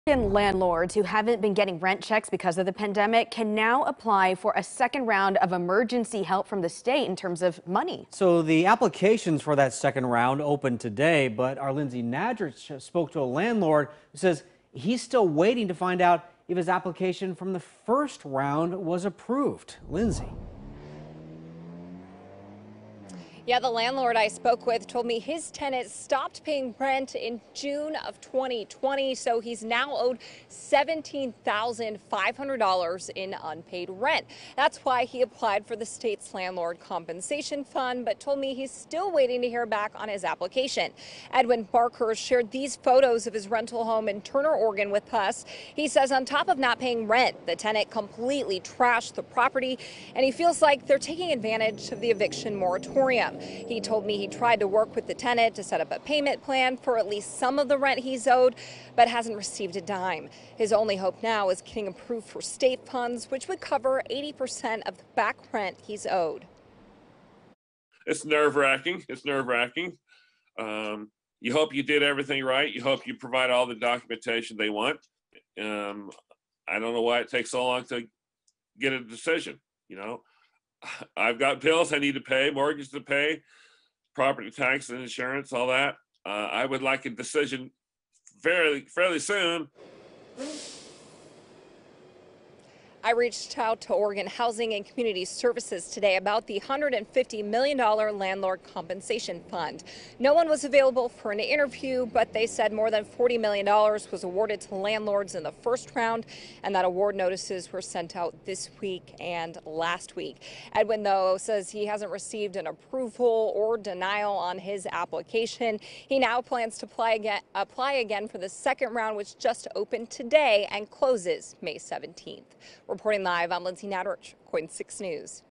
Apply now for 2nd round of Oregon landlord rent relief
Oregon landlords who haven’t been getting rent checks due to the pandemic can now apply for the second round of emergency funding from the state.